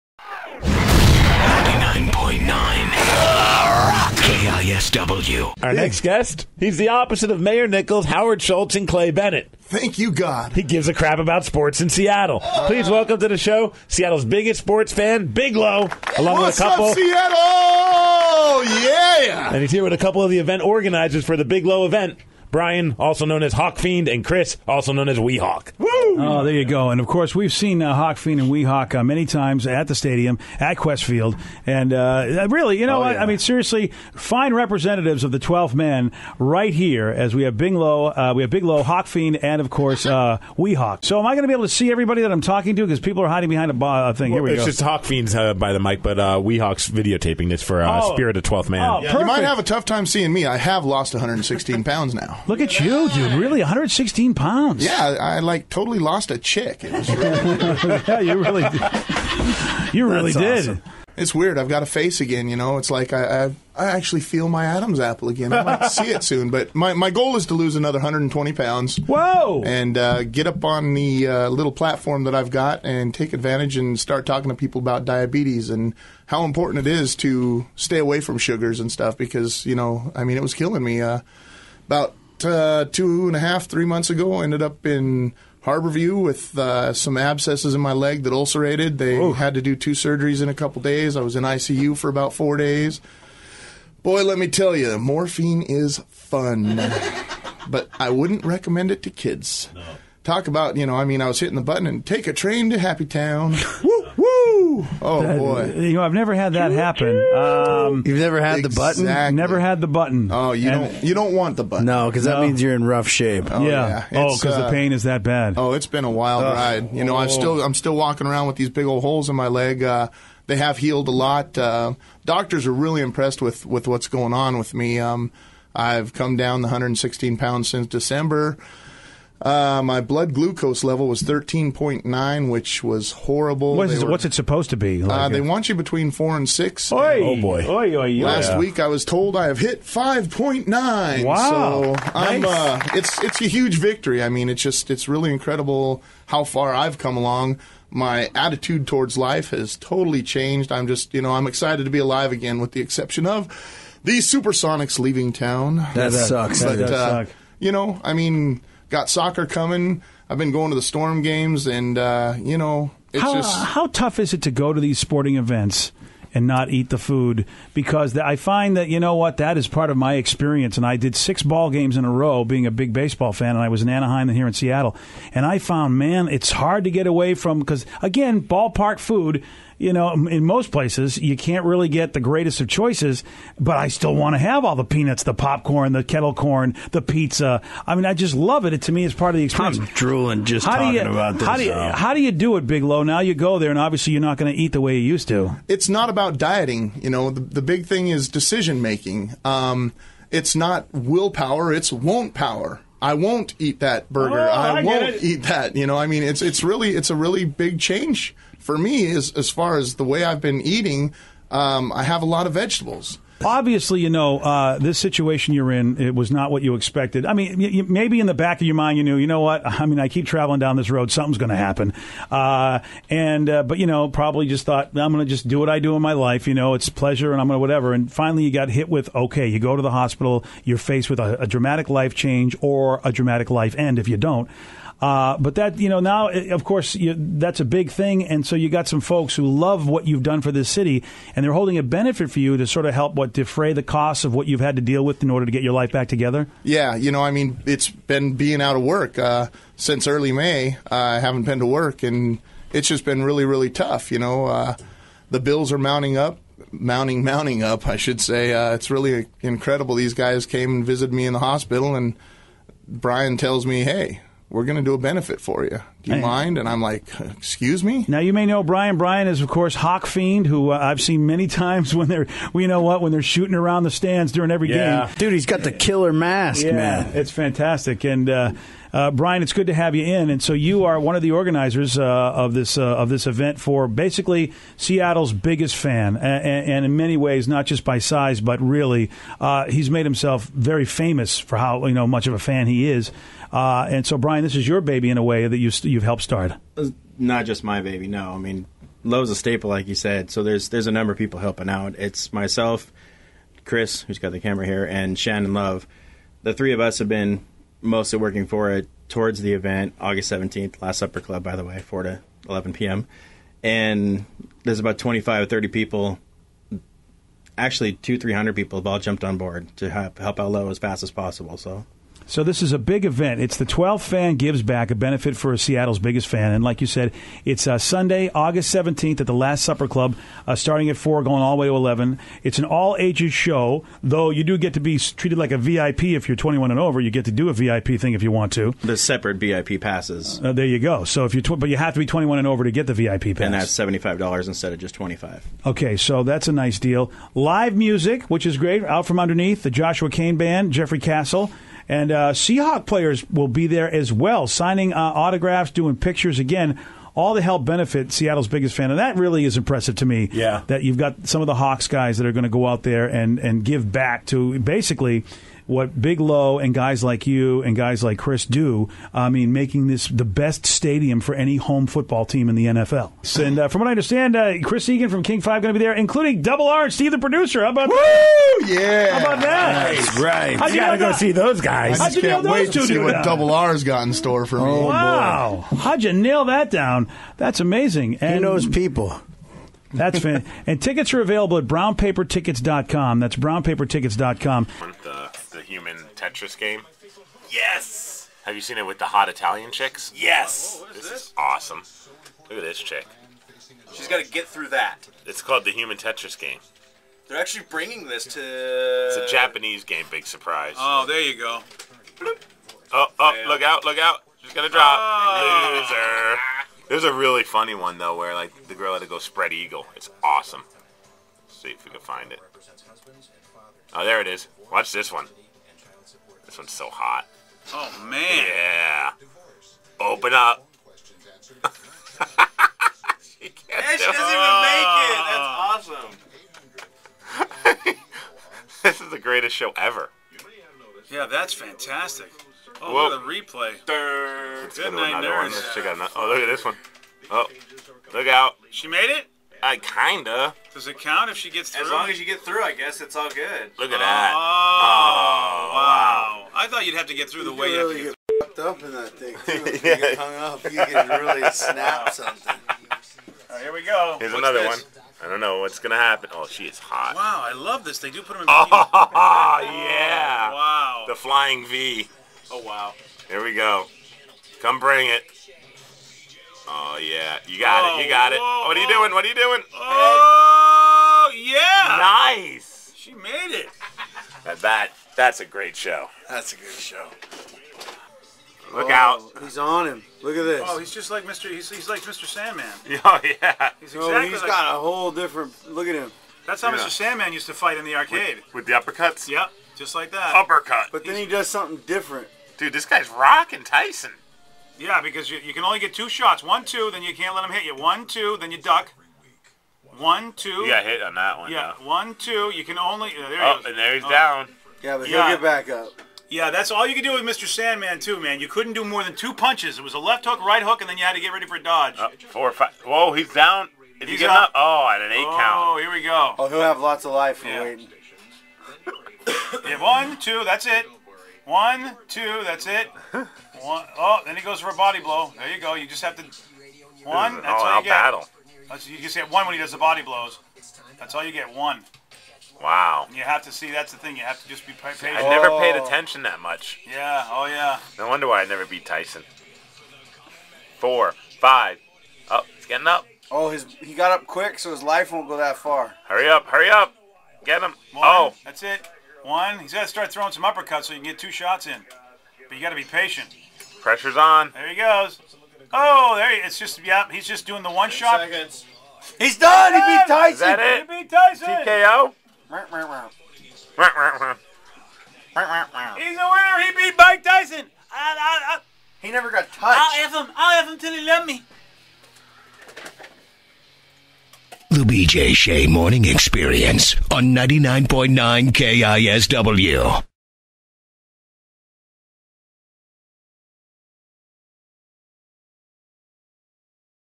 The BJ Shea Video Blog 07/29/08 Day 583 "Big Lo"
Today's Video Blog features Big Lo--Seattles Biggest Sports Fan.  Also in studio were Bryan a.k.a. HawkFiend, and Chris a.k.a. Wee Hawk.

We had them in studio because Big Lo has been battling diabetes, and some of his buds are putting on a benefit show to help pay some of the costs.

Also on the video blog we check out human tetris with hot Italian chicks, and we watch someone knock out Mike Tyson!